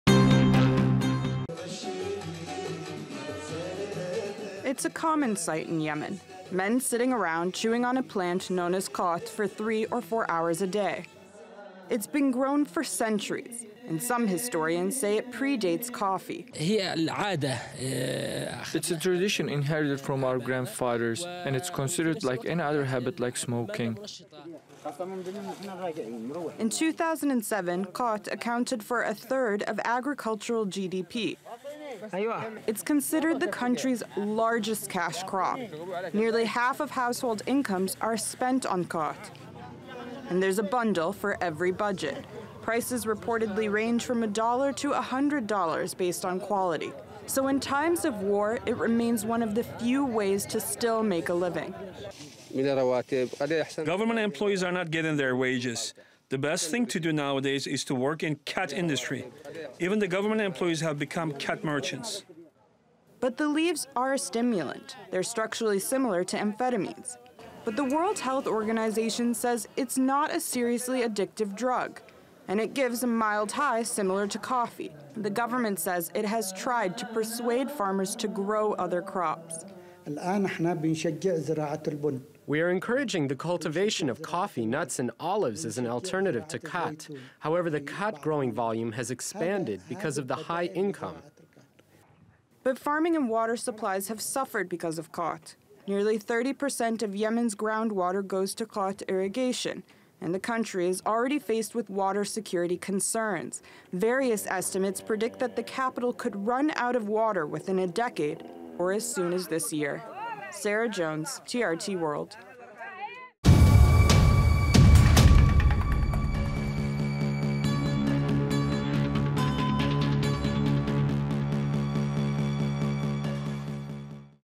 Money Talks: Khat business in Yemen
In Yemen, the war has driven hundreds of thousands of people to the brink of starvation. It's also halted exports, accelerated inflation and severely limited fuel imports. But people are still spending what little they have in this cash based economy. Nearly 90 percent of adult males, and 33% of women, in Yemen chew a stimulant known as khat. And while other industries are struggling, for khat sellers, it's business as usual. Sarah Jones reports.